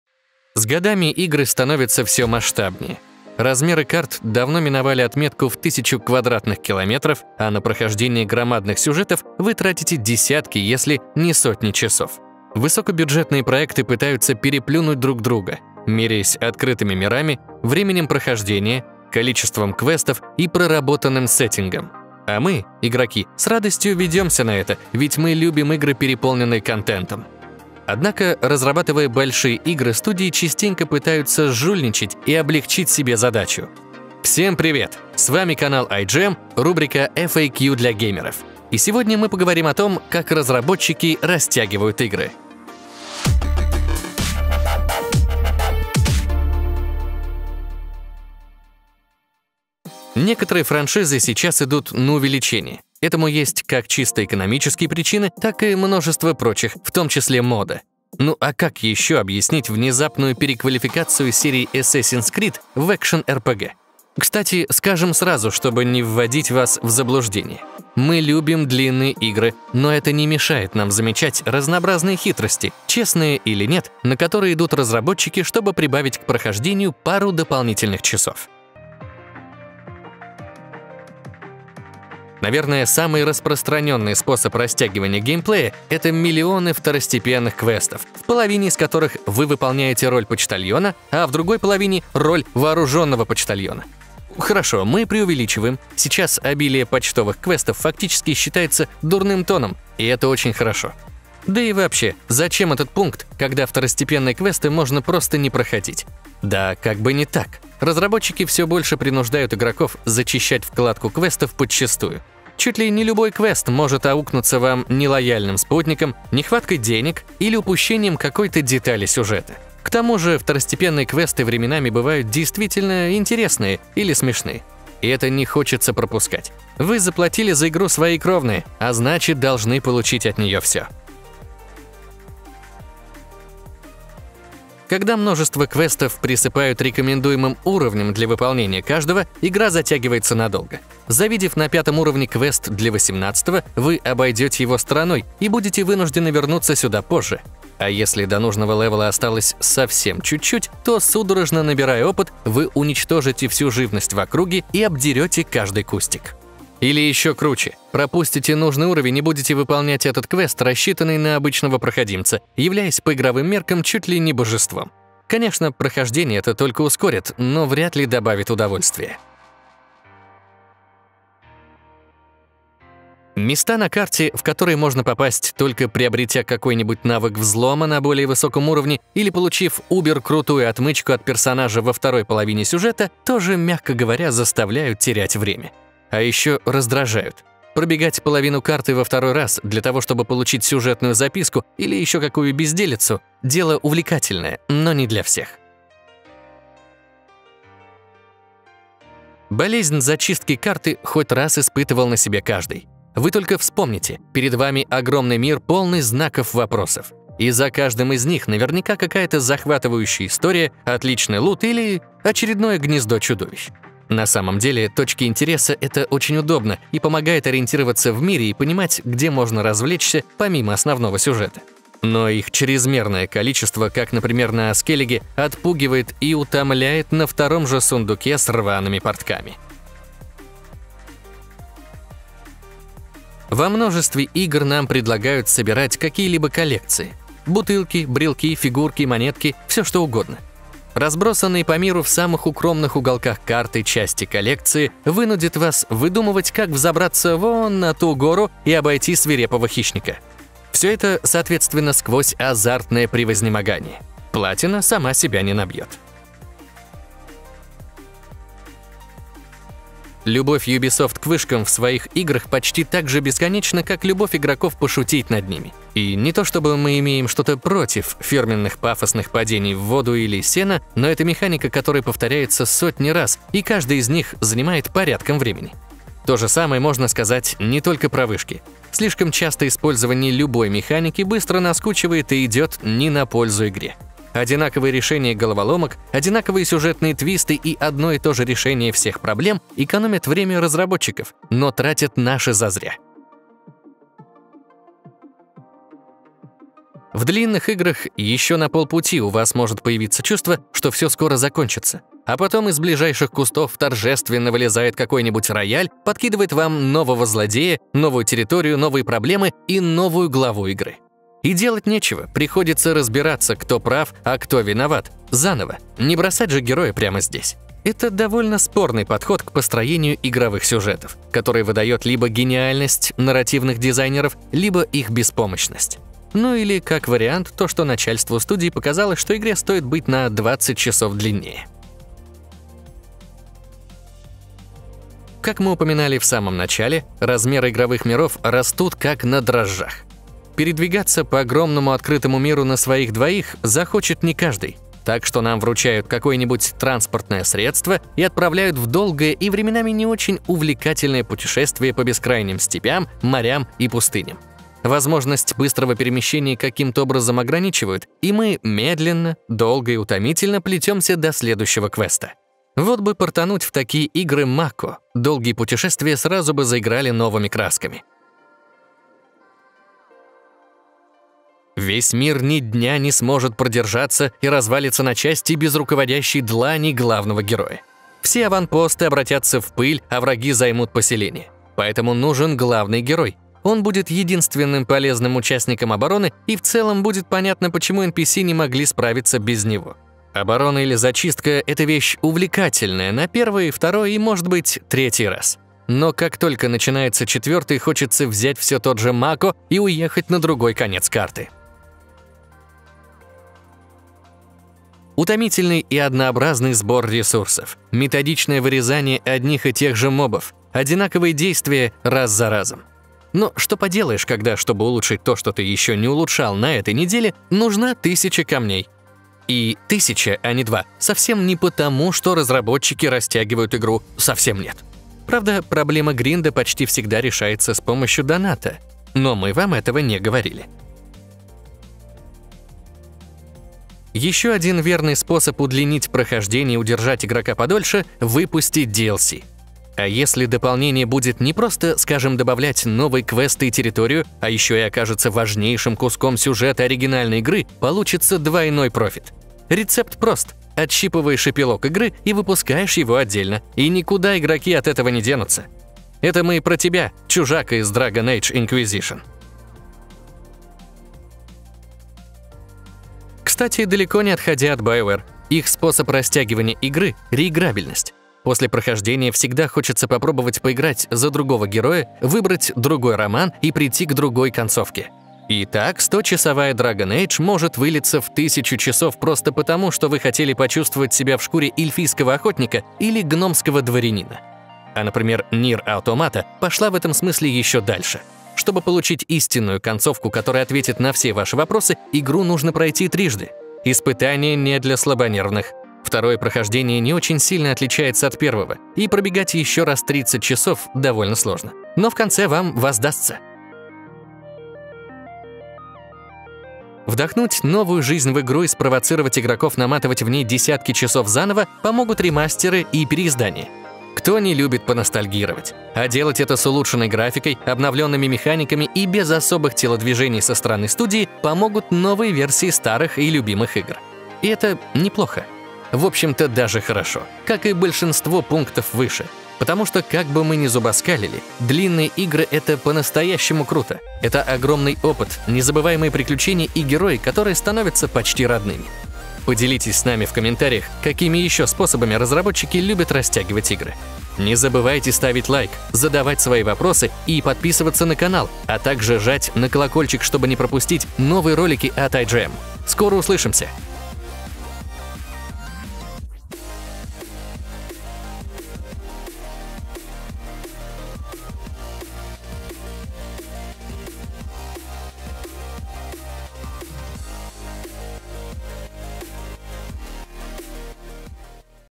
Как разработчики растягивают игры?
Способов, которыми разработчики растягивают свои игры, великое множество. Мы, чтобы не растягивать своё видео, взяли основные и рассказываем о них в этом ролике.